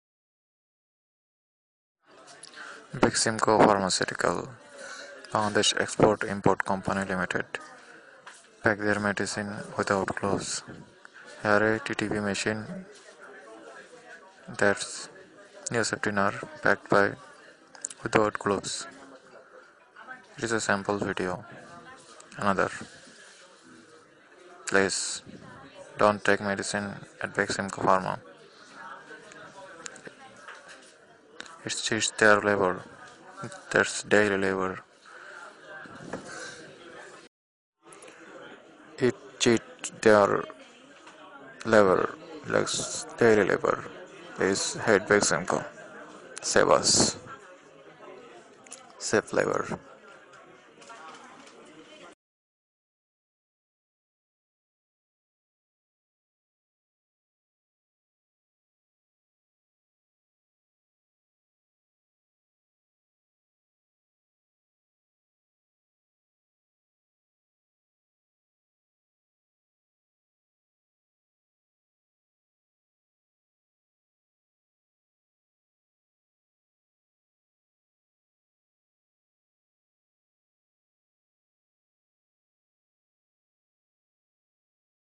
Pharmaceutical Manufacturing Company Beximco Pharmaceuticals Ltd.
Pharmaceutical Manufacturing Company Beximco Pharma pack medicine without gloves.